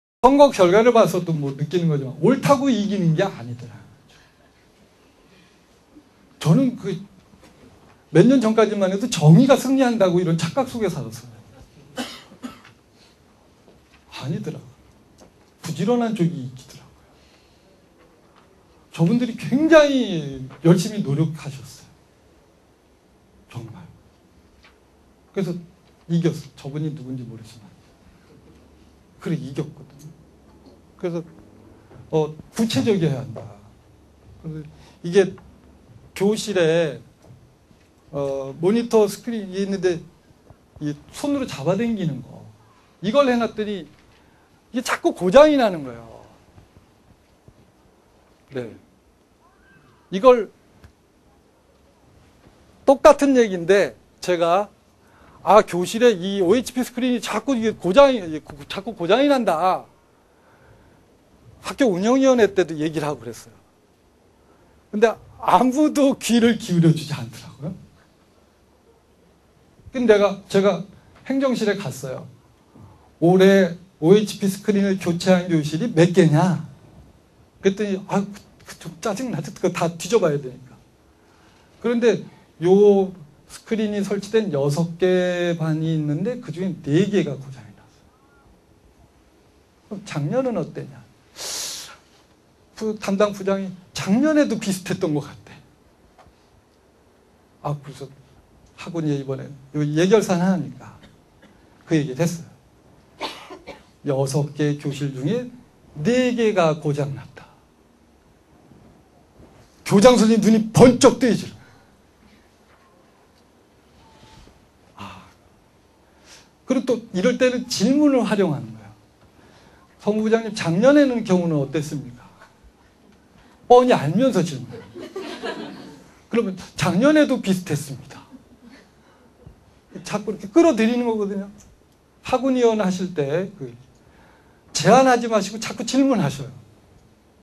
교원의 감정코칭: 교직사회 갈등해결법 9 옳되구체적이어야한다
참여소통교육모임 )직무연수 영상입니다.
강사 송형호